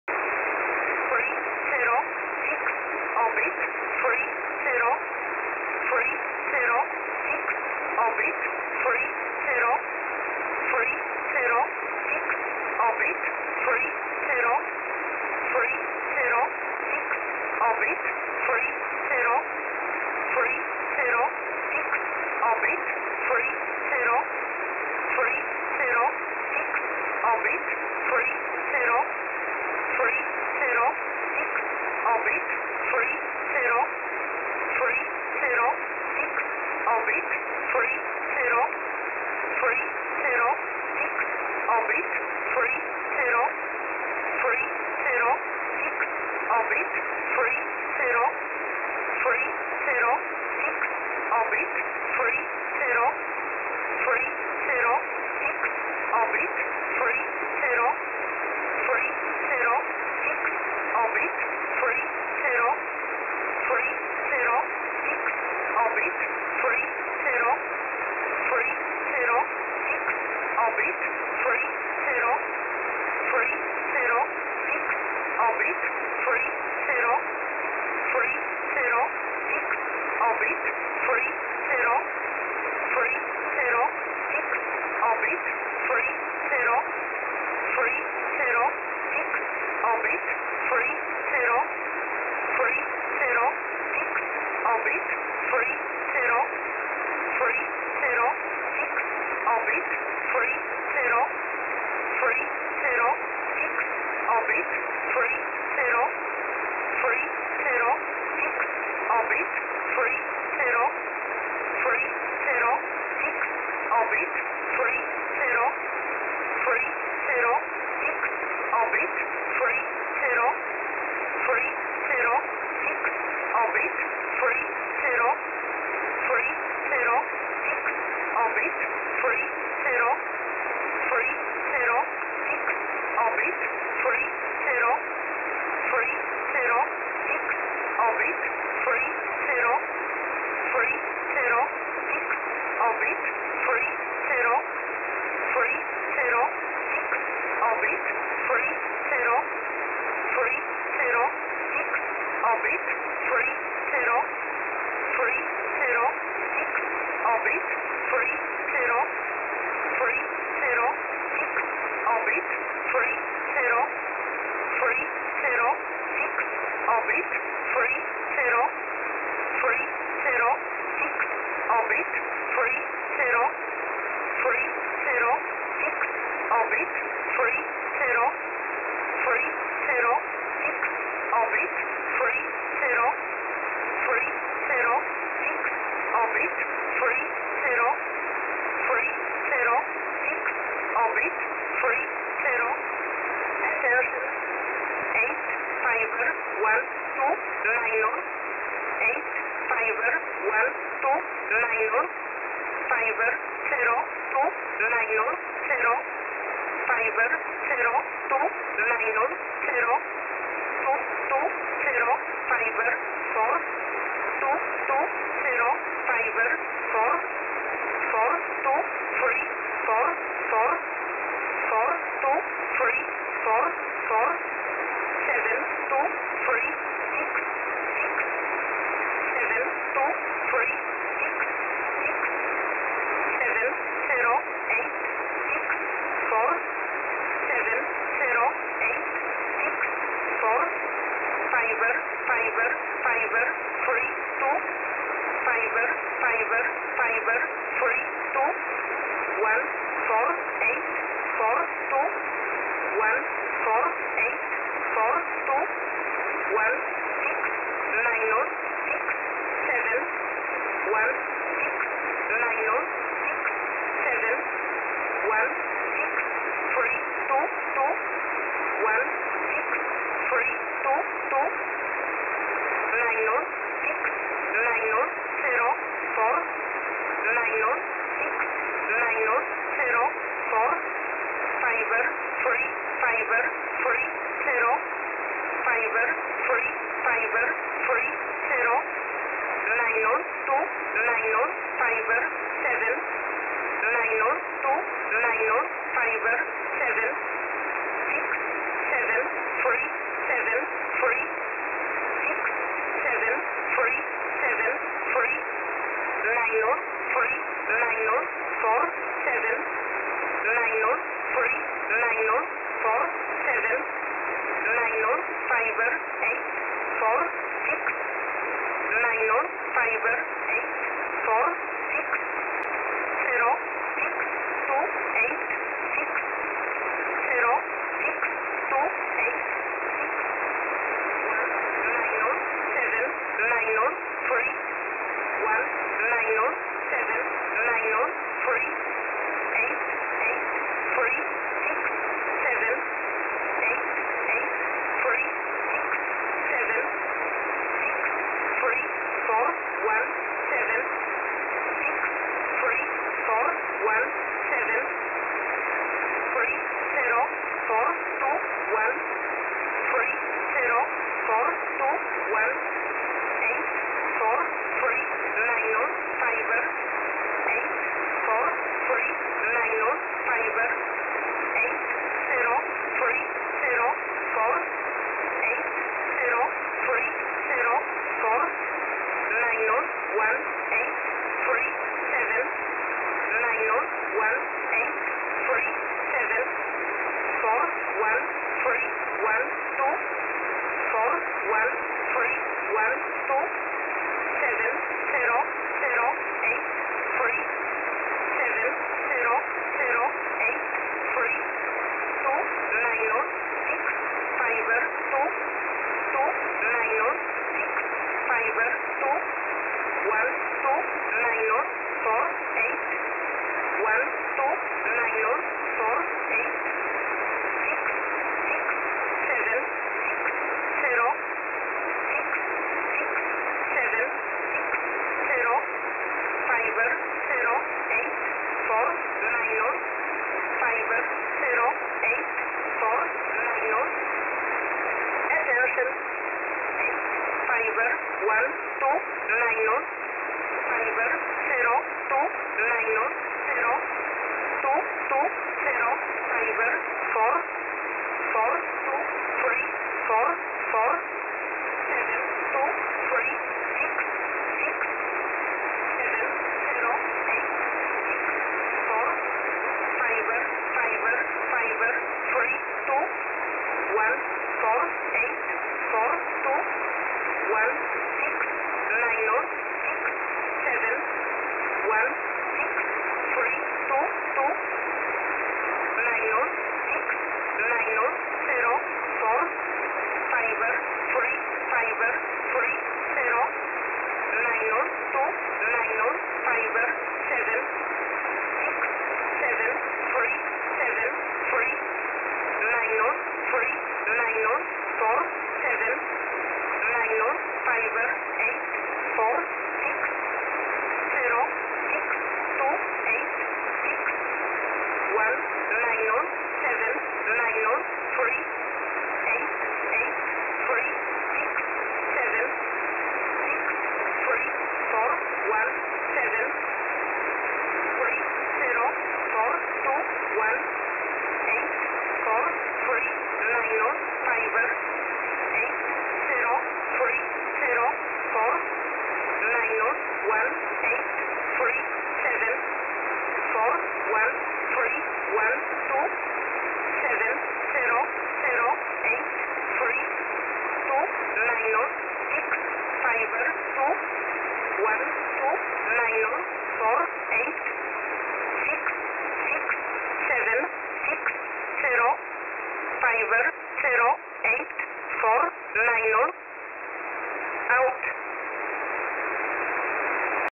E11a Message 02/05/2023 1000 UTC 12153kHz
Poland

Number Station

KiwiSDR

Shortwave Radio